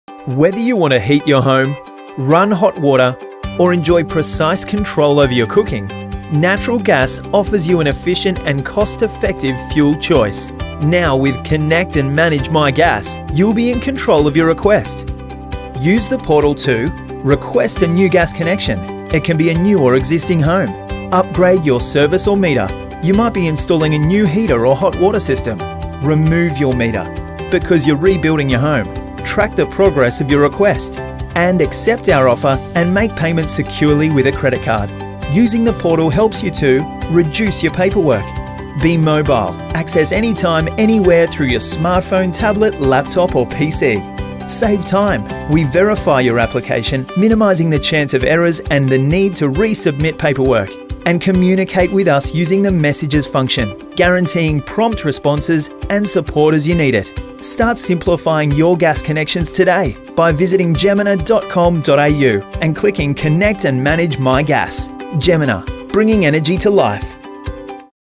Connect and manage your gas with Jemena
Do you live in NSW and want to connect to natural gas? Watch our video and learn how you can simplify your gas connection.